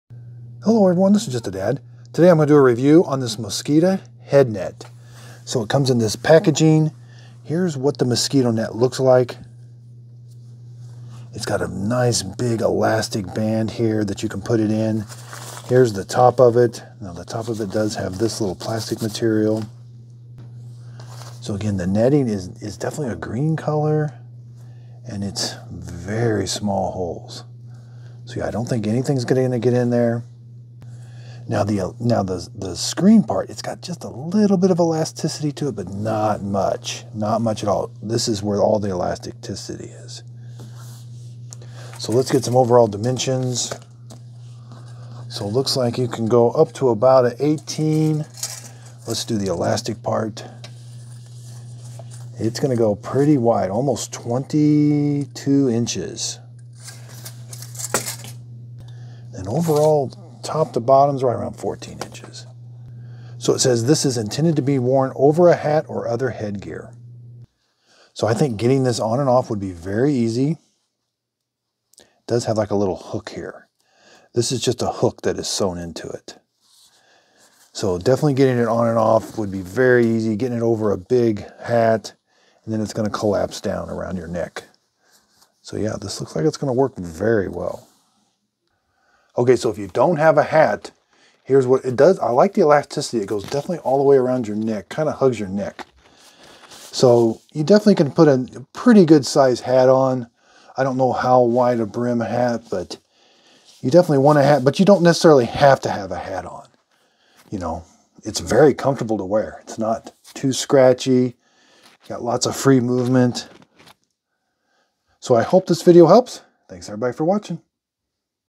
Review Coghlan's Mosquito Head Net Standard Edition that fits over hat
Coghlan's Mosquito Head Net standard edition How to use it and how big is the net.